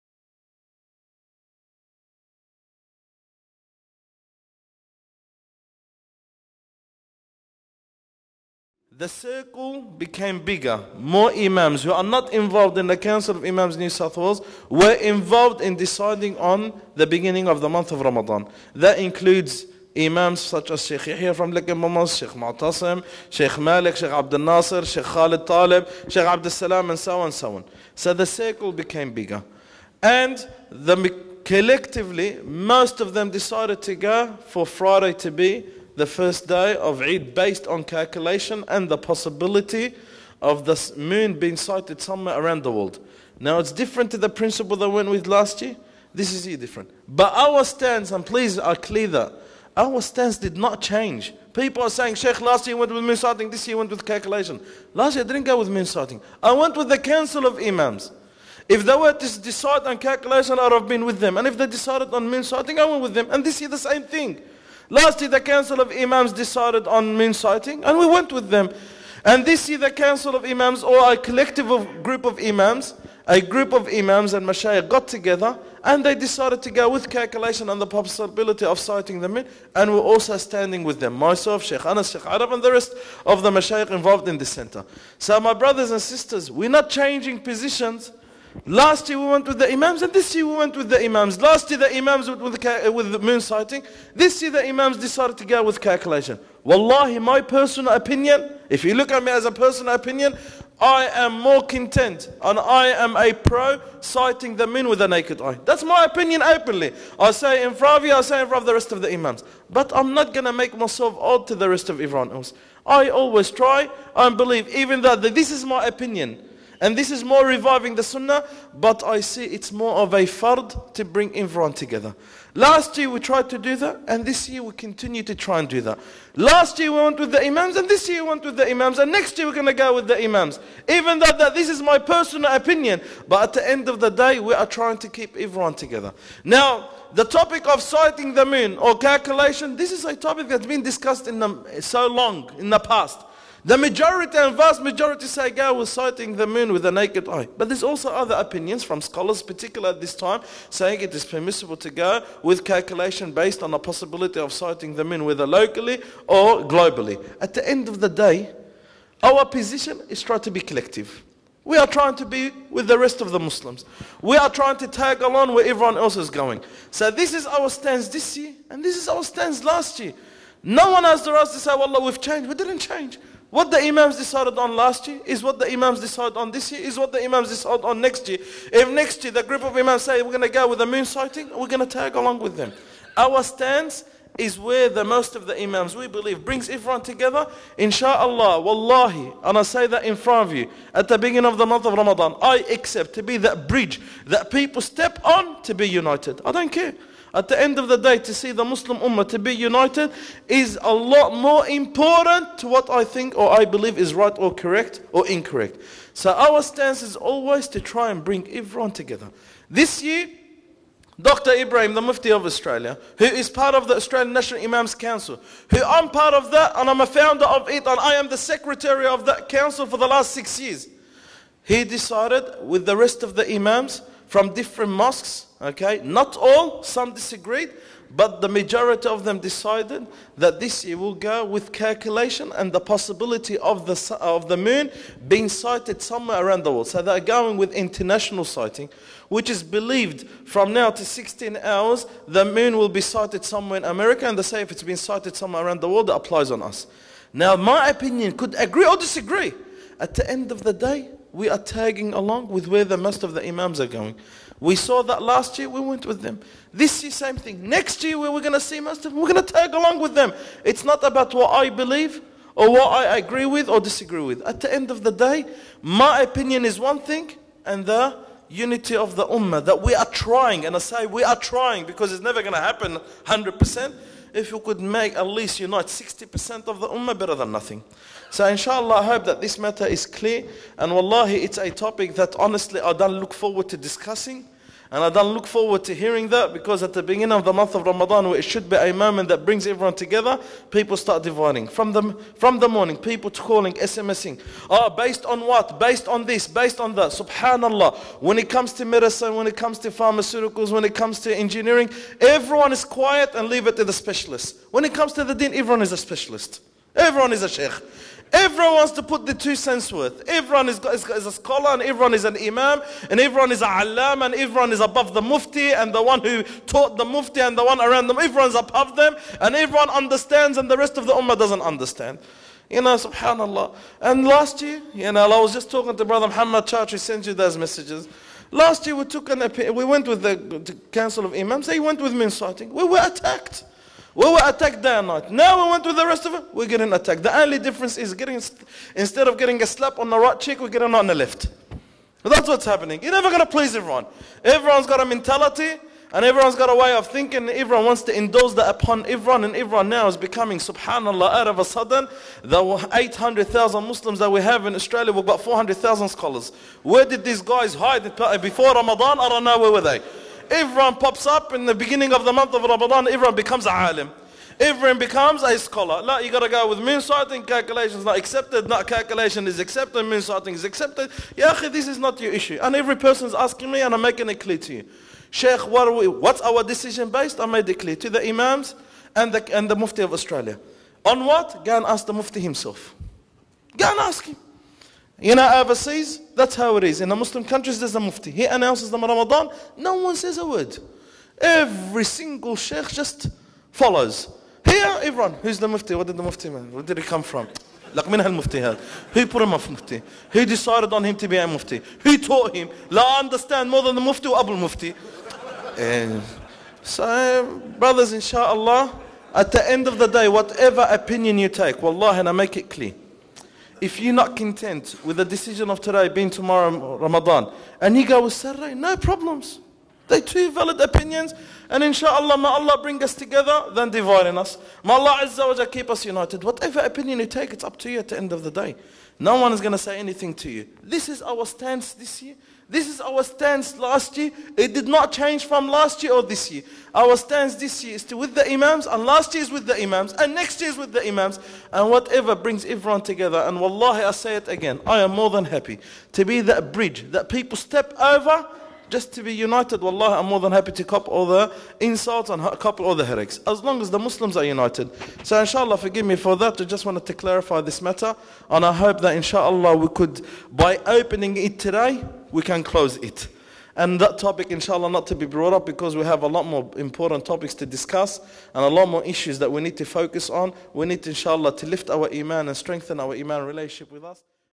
On Moon Sighting - Sh Shady Alsuleiman
Ramadhan Reminders - Short talks by various lecturers on different topics